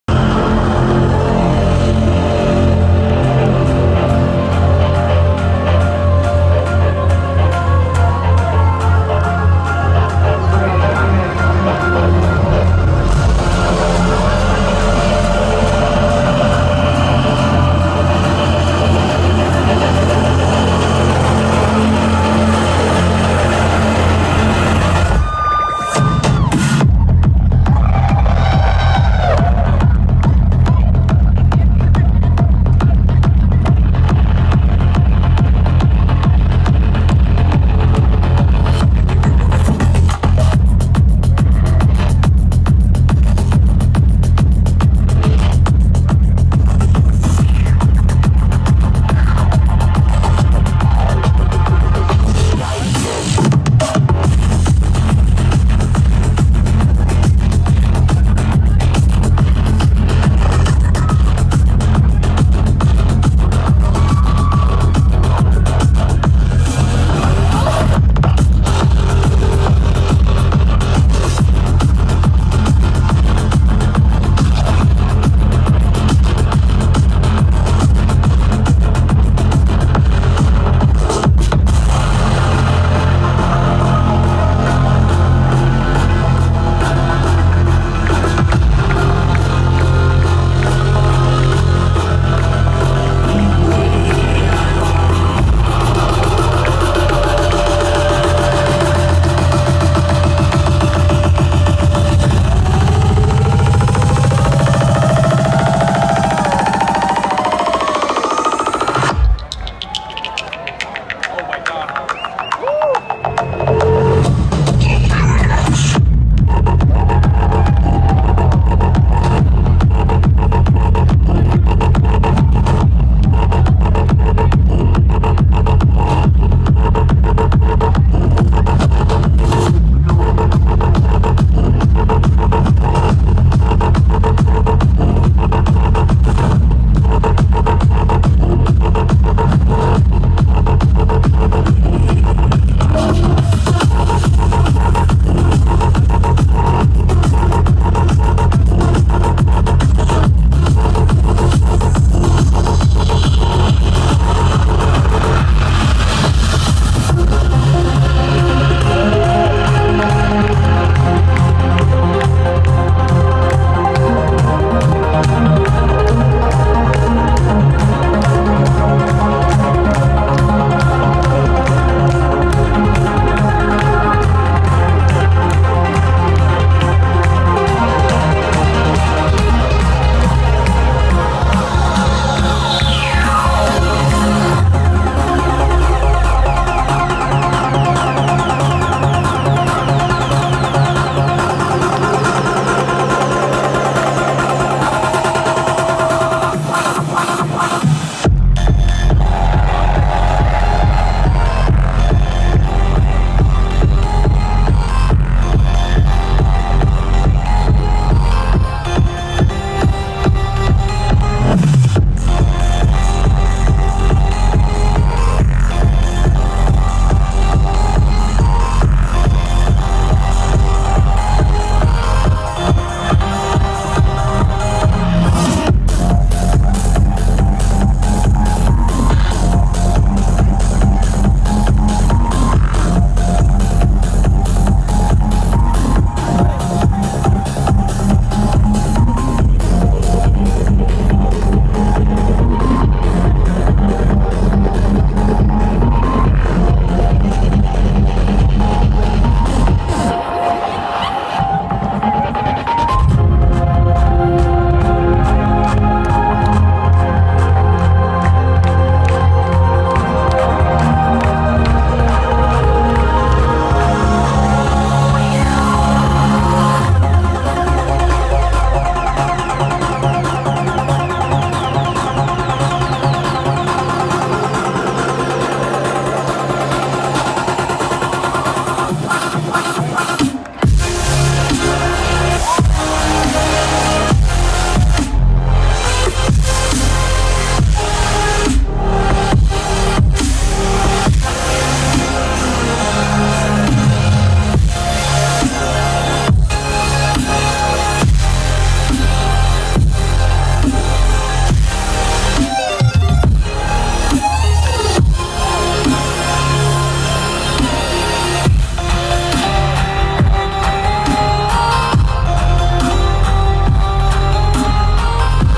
Seven Lions at Common Ground Miami
Seven Lions just killin it with the psy at Common Ground Miami! :D